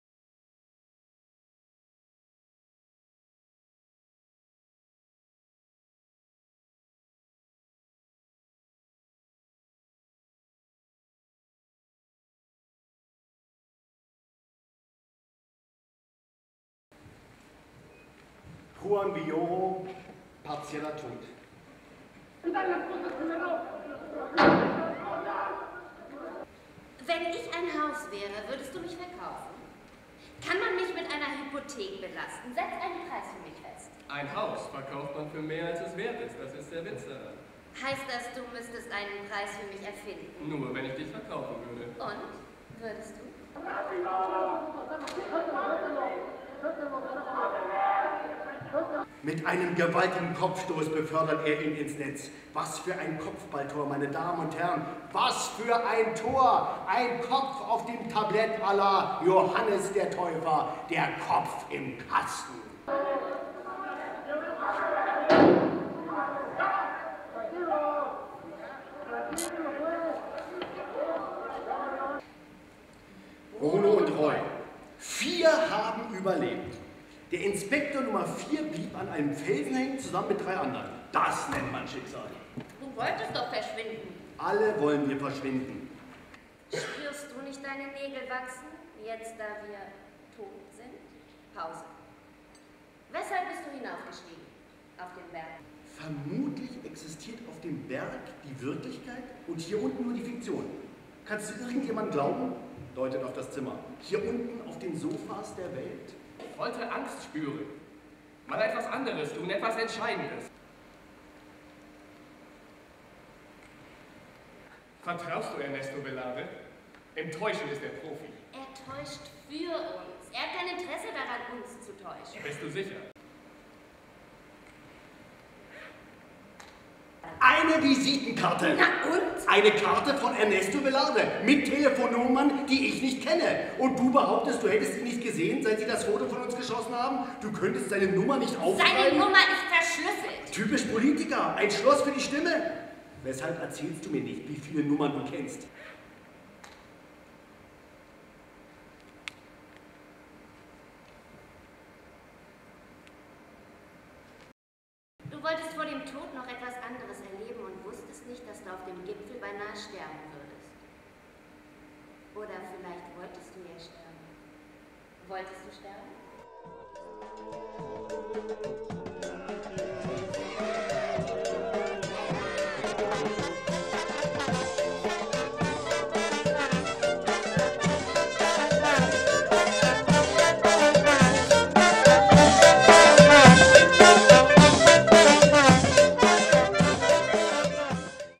Partieller Tod / Inszenierte Lesung
Premiere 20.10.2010 in der Staatsbibliothek zu Berlin
Autor: Juan Villoro
Übersetzung: Susanne Lange
Mit: Tobias Lehmann, Irma Mandler & Stephan Müller
Dramaturgie: Franziska Janetzky
Assistenz: Paul Kremershof & Isabelle Zinsmaier
Regie: Meret Kiderlen
Produktion: ARTOHOLICS e.V.